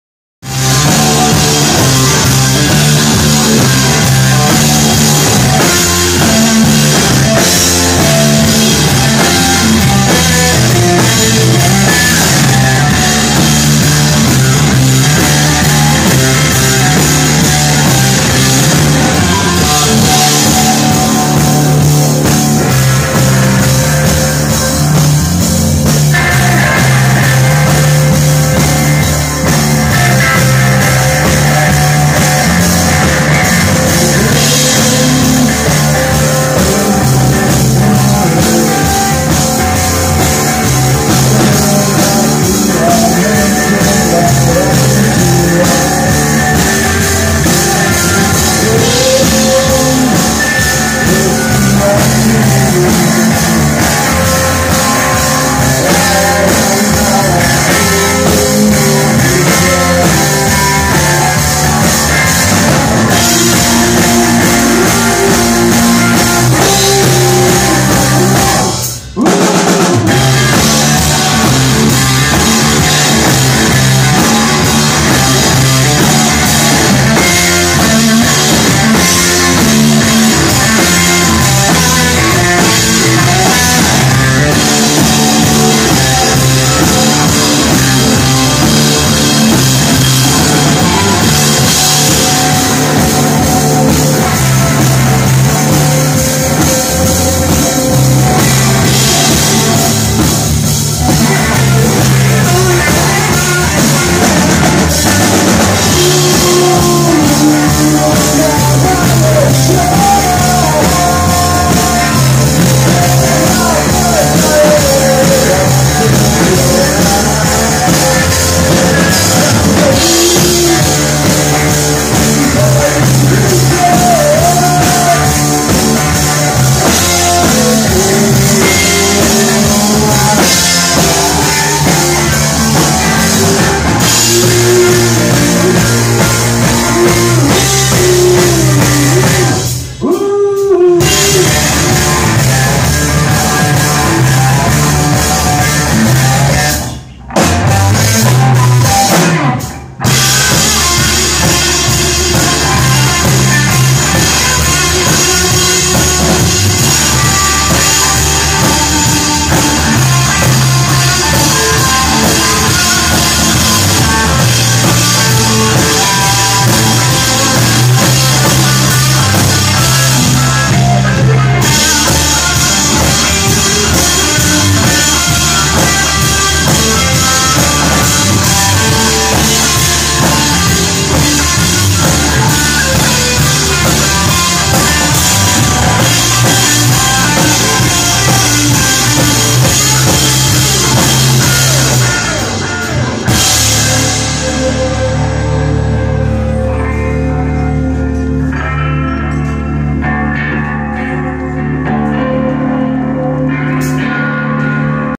Güacho @ Bar 227 31.05.15
Headlining act on a long Sunday night full of psychedelia.